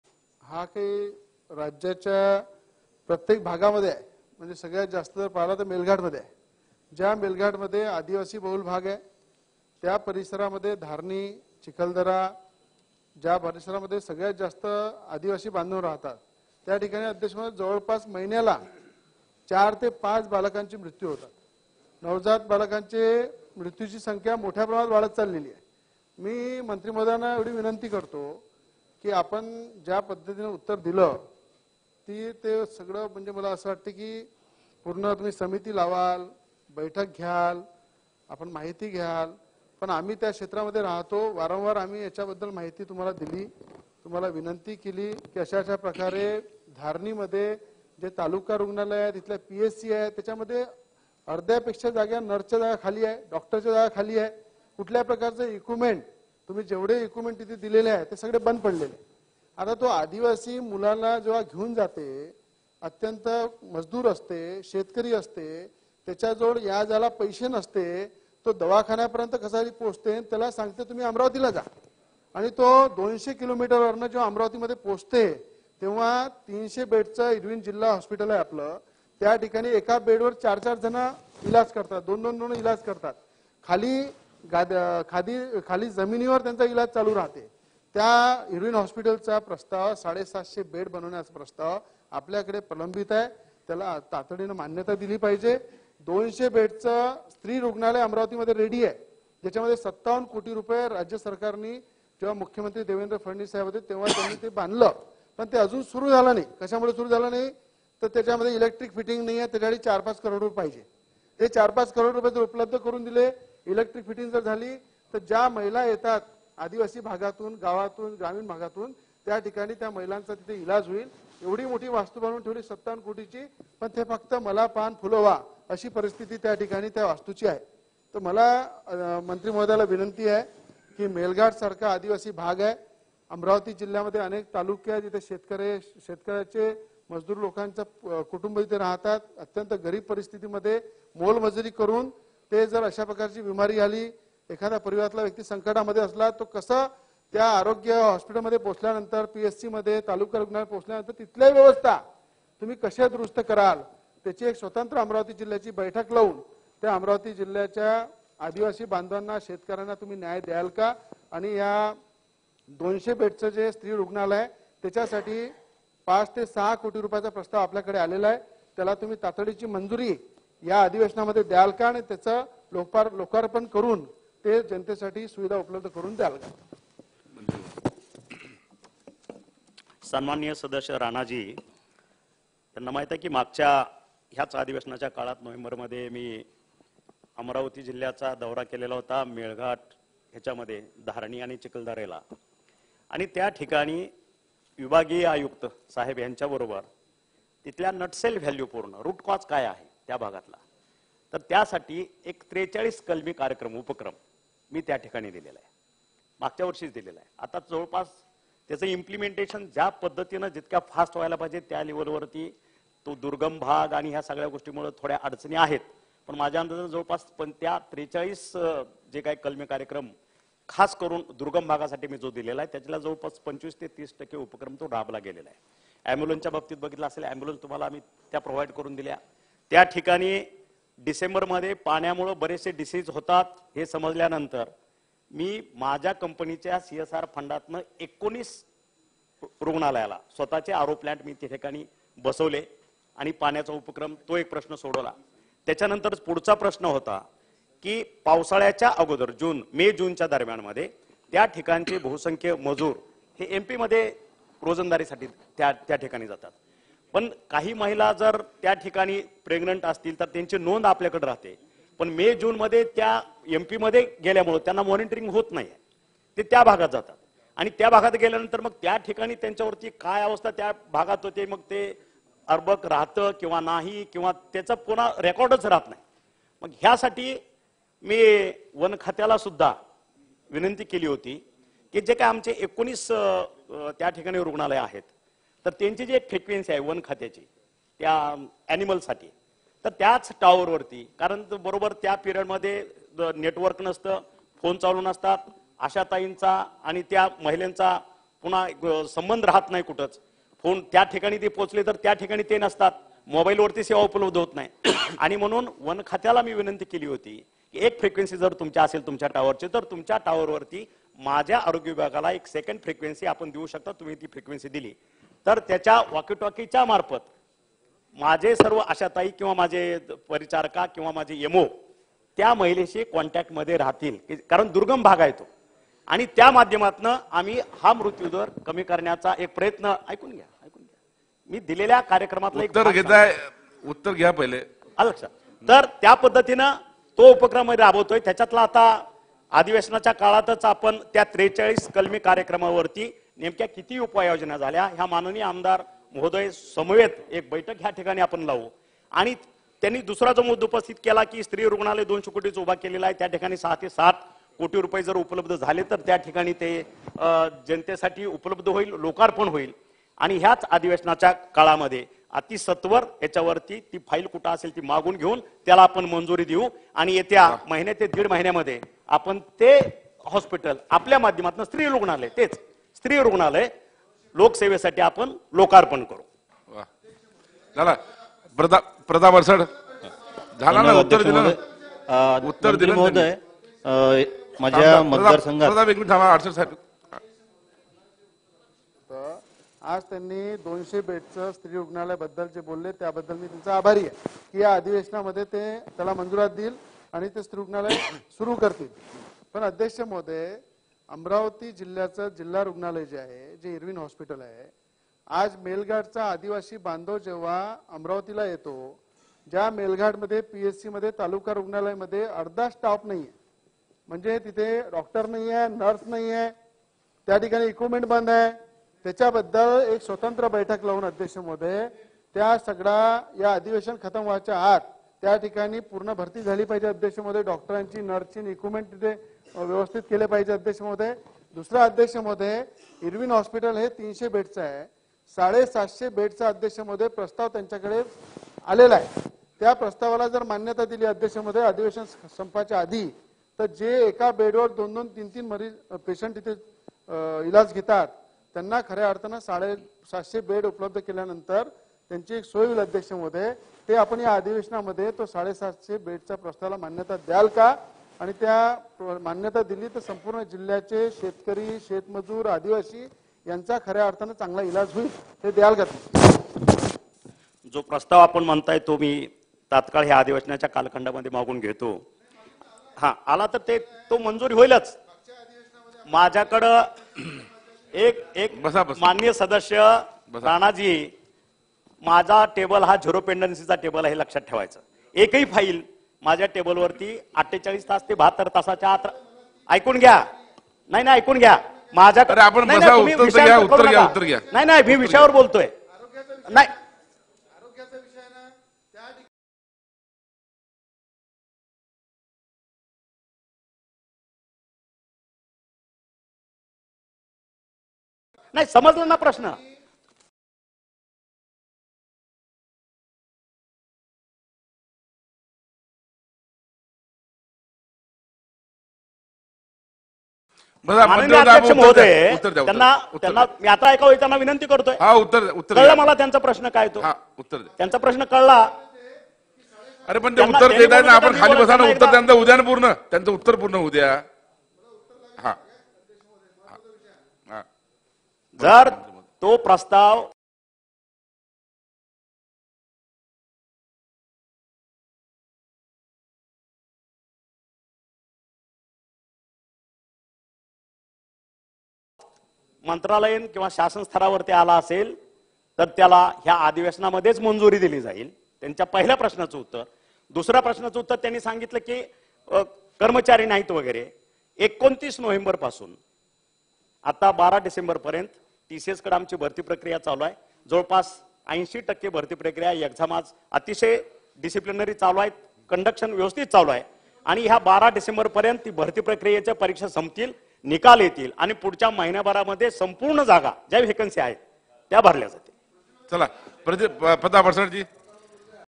मेळघाटतील आदिवासींच्या ,आरोग्य समस्यांवर आ. राणांनी विधानसभेत केली चर्चा
मेळघाट आदिवासी बहुल भाग, इर्विन बेडची संख्या वाढवणे,आरोग्य समस्यावर आ. राणांनी विधानसभेत केली चर्चा